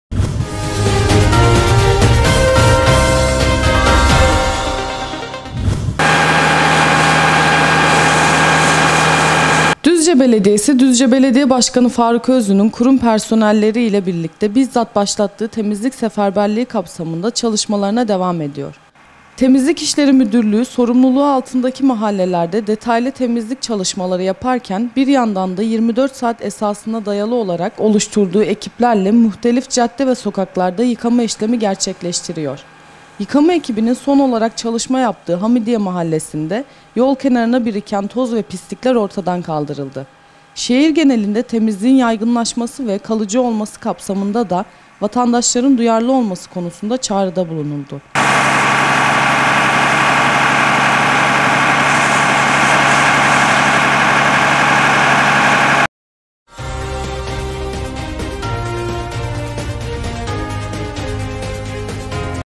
TEMİZLİK ÇALIŞMALARI DEVAM EDİYOR
Düzce Belediyesi, Düzce Belediye Başkanı Faruk Özlü'nün kurum personelleri ile birlikte bizzat başlattığı temizlik seferberliği kapsamında çalışmalarına devam ediyor. Temizlik İşleri Müdürlüğü, sorumluluğu altındaki mahallelerde detaylı temizlik çalışmaları yaparken bir yandan da 24 saat esasına dayalı olarak oluşturduğu ekiplerle muhtelif cadde ve sokaklarda yıkama işlemi gerçekleştiriyor. Yıkama ekibinin son olarak çalışma yaptığı Hamidiye mahallesinde yol kenarlarına biriken toz ve pislikler ortadan kaldırıldı. Şehir genelinde temizliğin yaygınlaşması ve kalıcı olması kapsamında da  vatandaşların duyarlı olması konusunda çağrıda bulunuldu.